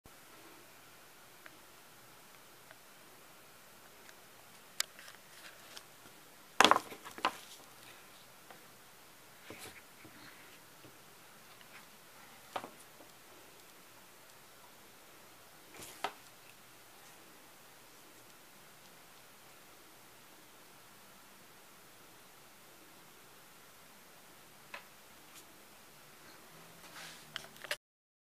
The Magic Seven Color Sword/Sabor Prototype Light Up Toy
This is the Magic Seven Color Sword/Sabor prototype light up toy from the Amazing Magical Adventures of Zuhan